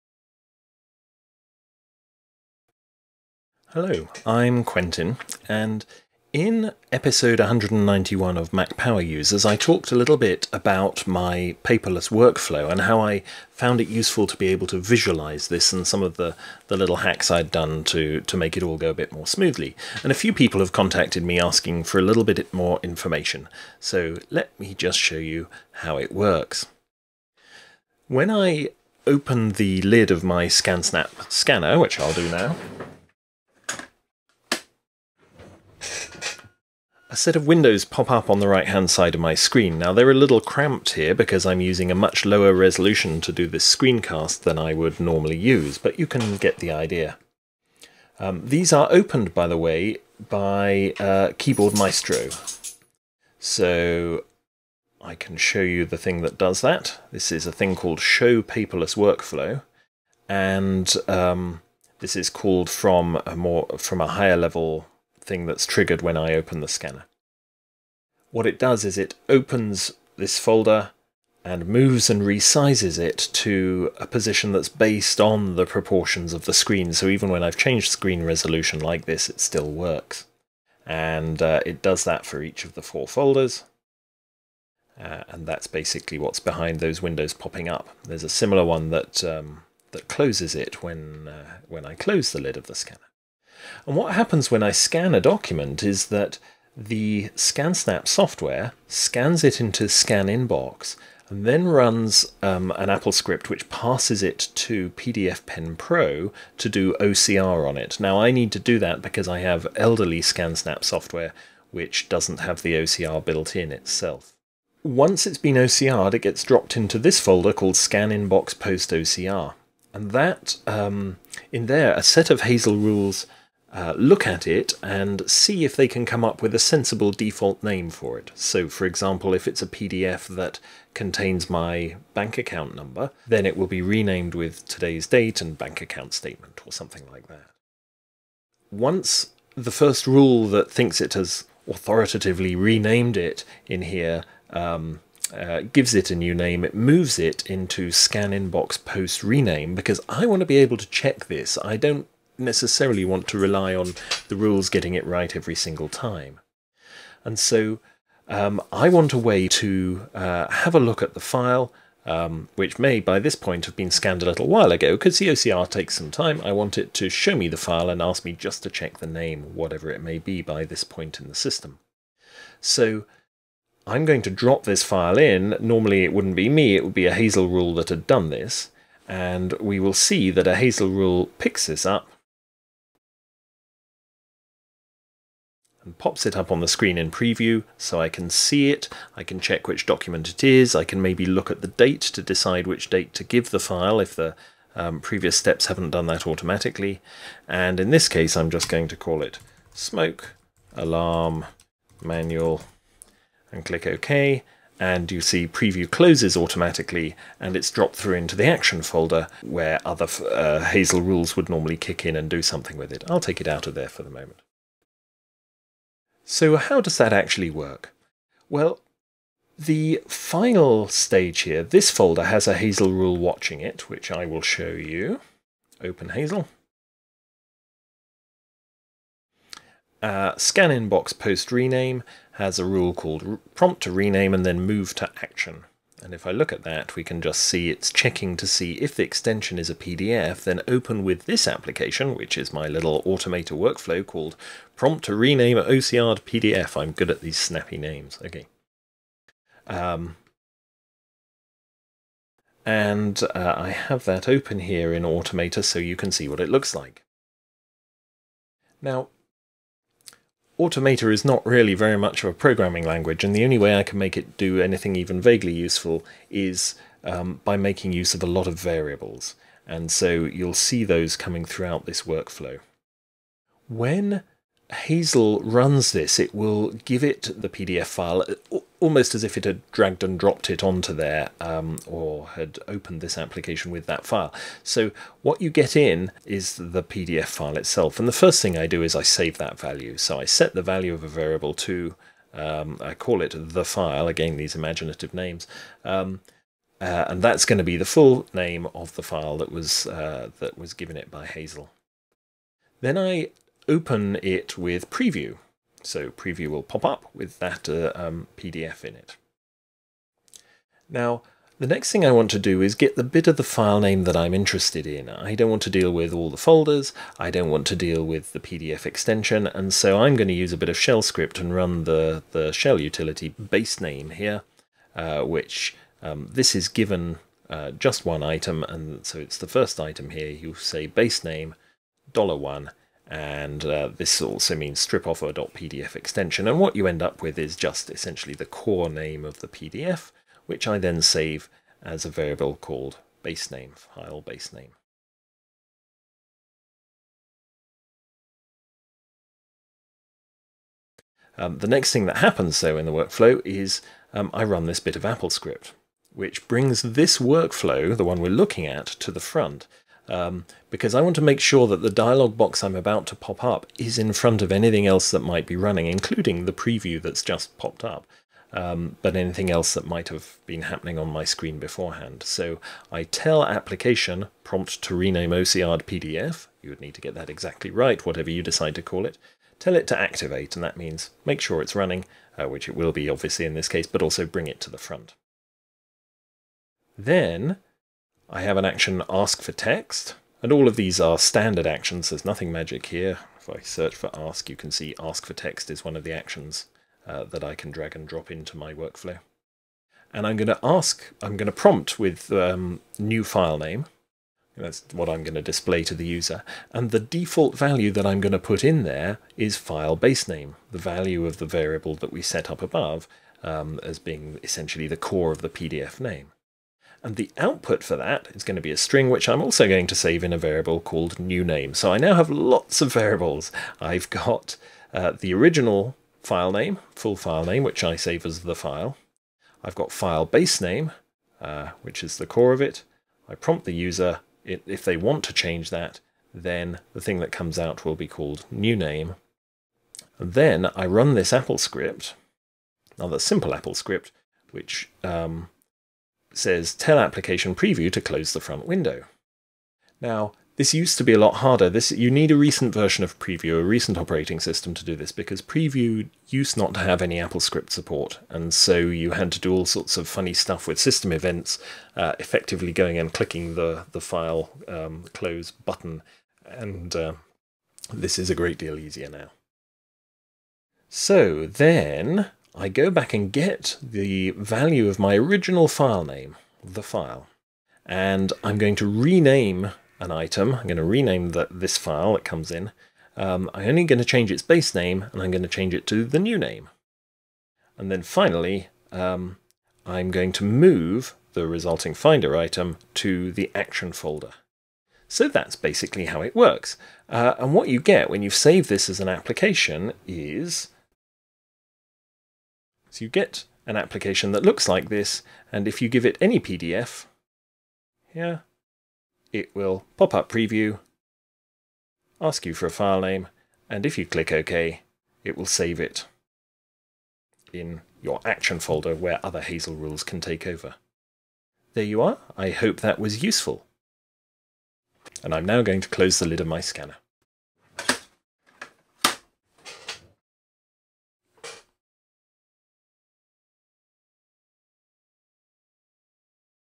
Visualising your Paperless Workflow
Some tips for Mac users, based on Keyboard Maestro, Hazel, and Automator

First mentioned as a brief audio clip in episode 191 of Mac Power Users.